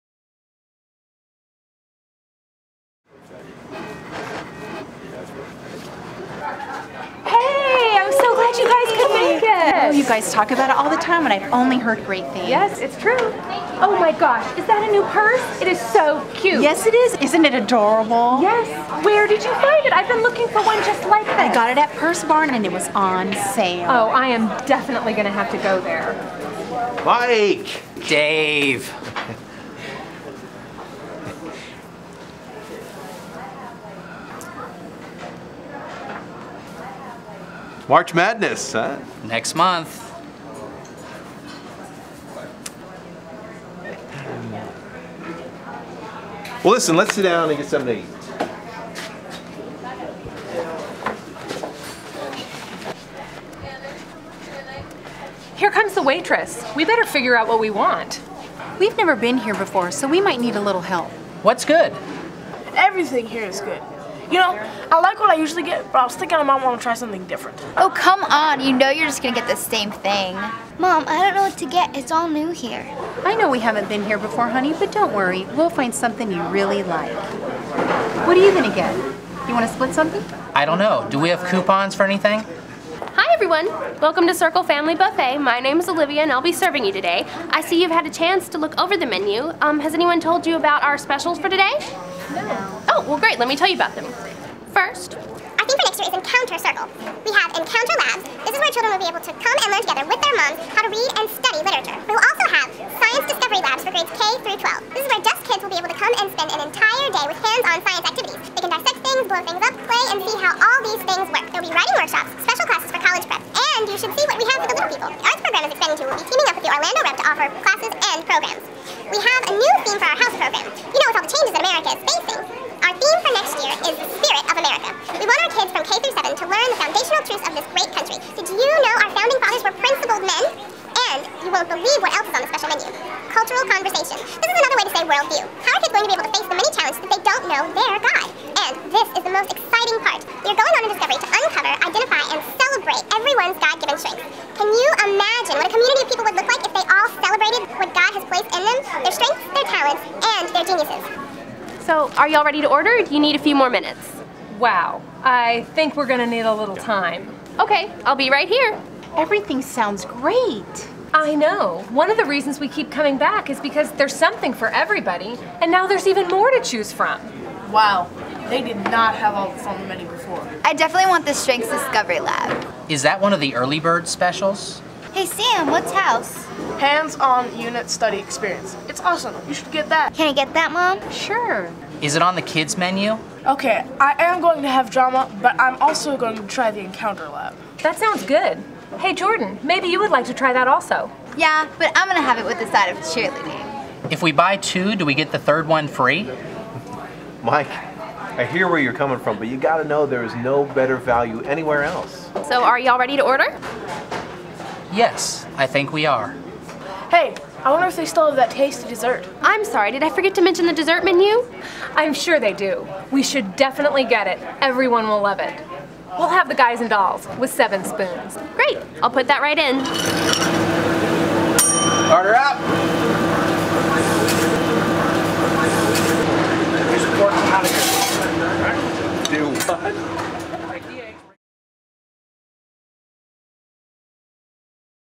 The Circle Diner
This video seeks to lightheartedly explain the bevy of new offerings at Circle Christian School for the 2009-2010 school year.  Cast: Jo Stroup, Mike Stroup, Jordynn Stroup, Tristynn Stroup, Olivia Richardson, Sam Theriault, Erin Theriault, & David Theriault.  Taped at the Osteen Diner in Osteen, FL